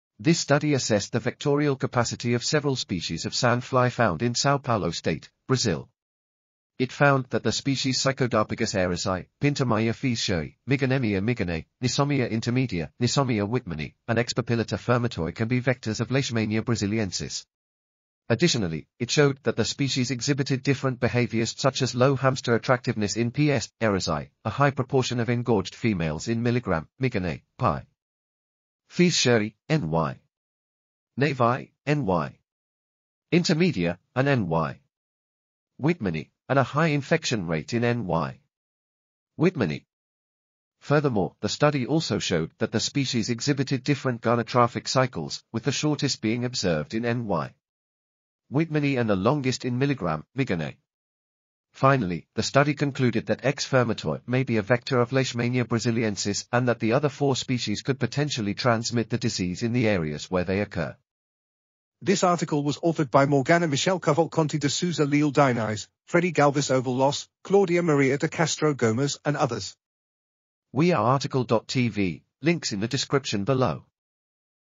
Host-biting rate and susceptibility of some suspected vectors to Leishmania brazilien... | RTCL.TV
### Keywords ###

### Article Attribution ###
Title: Host-biting rate and susceptibility of some suspected vectors to Leishmania braziliensis
Authors: Morgana Michele Cavalcanti de Souza Leal Diniz, Fredy Galvis Ovallos, Claudia Maria de Castro Gomes, Cecilia de Oliveira Lavitschka ,and Eunice Aparecida Bianchi Galati
Publisher: BMC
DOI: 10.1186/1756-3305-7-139

### Video Timestamps ###
0:00:00 - Summary
0:01:01 - Title
0:01:10 - Outro
0:01:14 - End